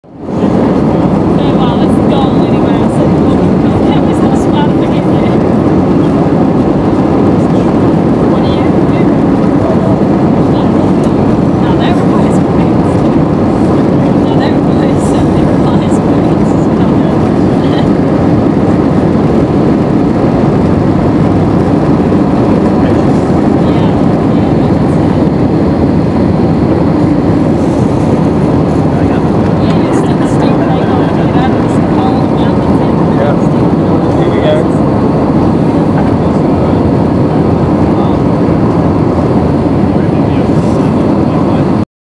Air New Zealand Scary Take off from Queenstown Airport
Beautiful aerial view of Queenstown from Air New Zealand Airbus A320 taking off from Queenstown Airport